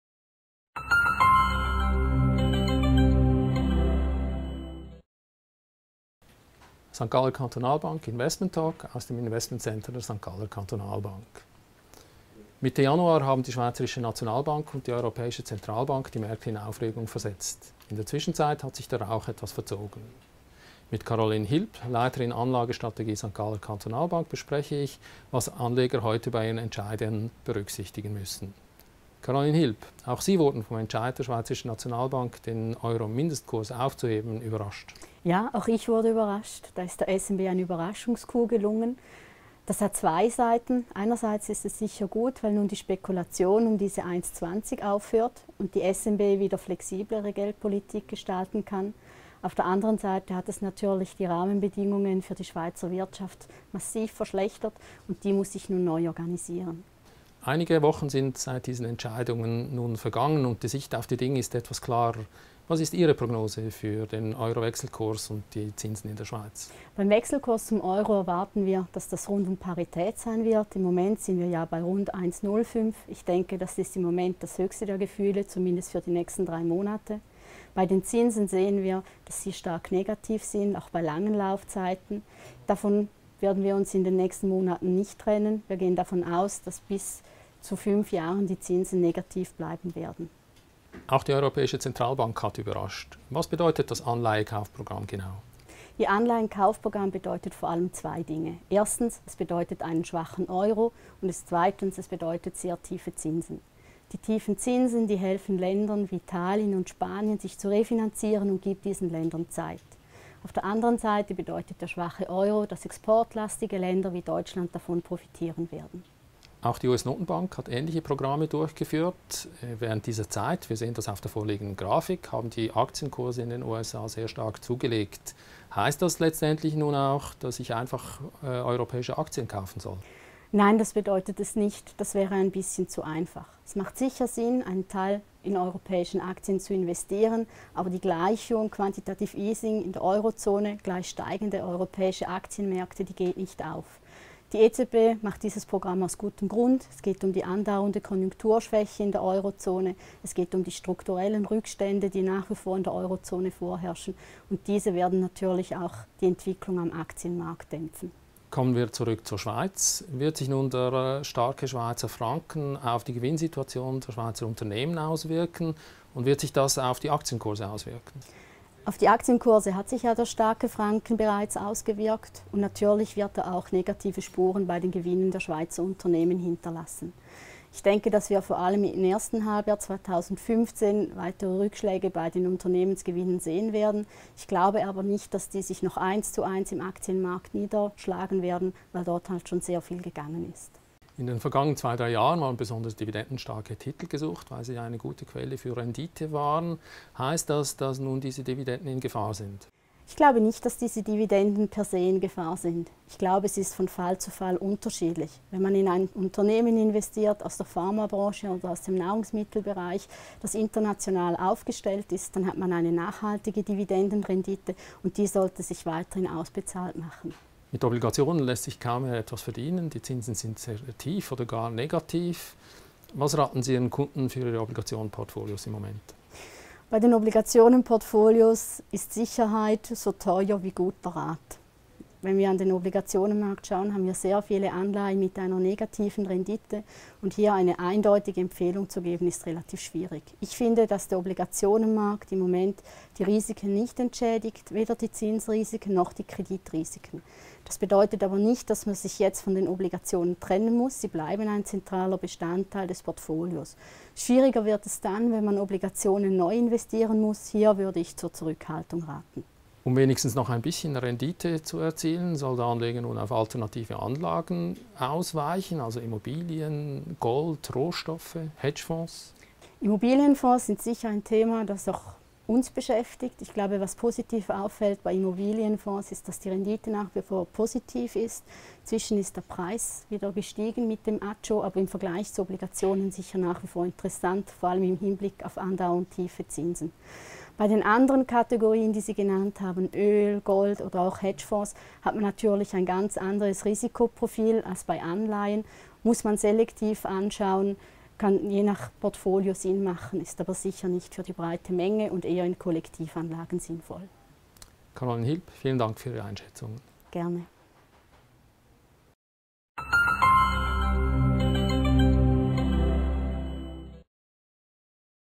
SGKB Investment Talk: Investing with a strong Swiss franc
Caroline Hilb Paraskevopoulos, head of investment strategy at St.Galler Kantonalbank, gives her analysis of financial markets in the wake of the decisions taken by the Swiss National Bank (SNB).